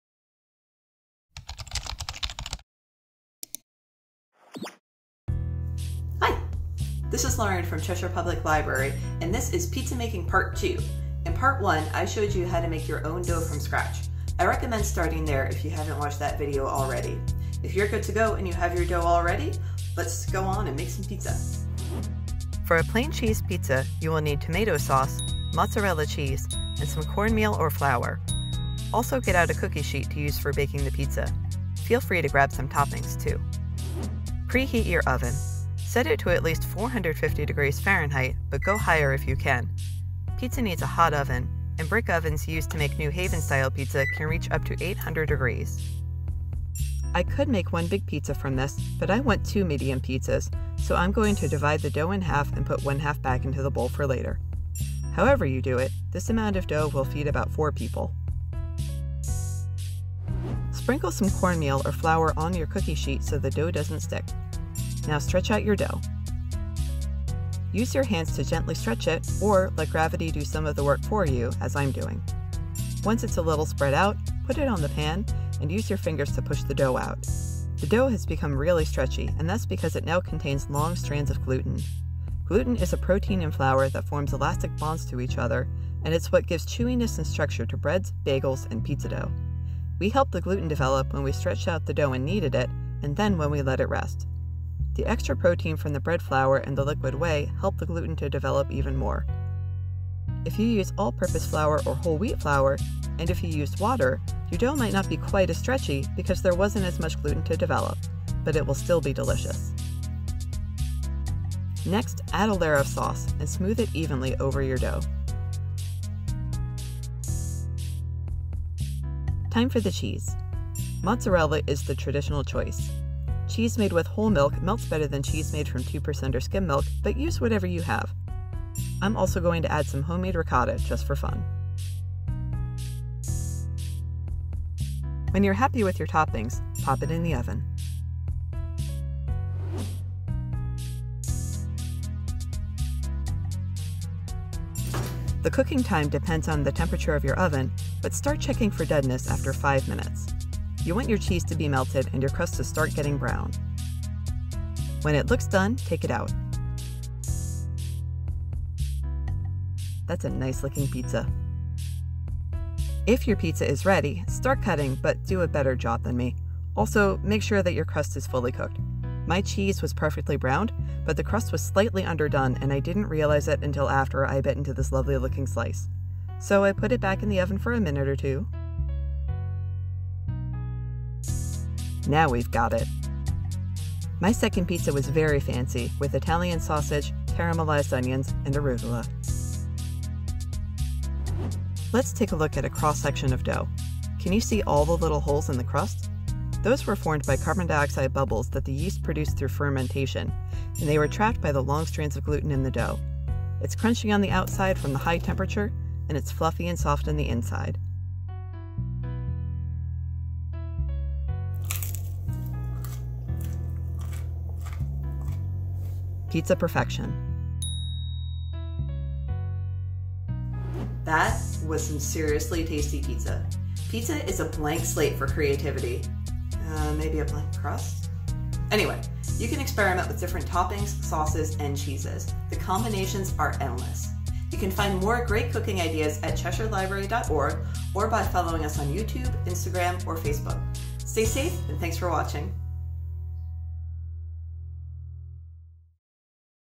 Make Your Own Pizza, Part 2: Building the Perfect Pie
Lauren from Cheshire Library continues her pizza making tutorial.  In part 1 we learned to make the dough. Now we'll build the perfect pie!

For a basic cheese pizza, you'll need tomato sauce, mozzarella cheese, a little cornmeal or flour, and a cookie sheet to use for baking the pizza on. Feel free to add whatever other toppings you like, too.

Preheat your oven. Set it to at least 450 degrees Fahrenheit, but go higher if you can. Pizza needs a hot oven, (brick ovens used to make New Haven style pizza can reach up to 800 degrees!)

Sprinkle some cornmeal or flour on your cookie sheet so the dough doesn’t stick. The dough recipe from Part 1 will yield enough dough to make 1 jumbo pizza or 2 medium pizzas. Divide the dough in half if you are making 2 pizzas.  Stretch out your dough. Use your hands to gently stretch it, then put it on the pan and use your fingers to push the dough out toward the edges.

Next, add a layer of sauce, and smooth it evenly over your dough.
Add your cheese and any other toppings, and pop it into the oven. The cooking time depends on the temperature of your oven, but start checking for doneness after five minutes or so. You want your cheese to be melted and your crust to start getting brown.

Let the cooked pizza sit for a minute before slicing. Enjoy!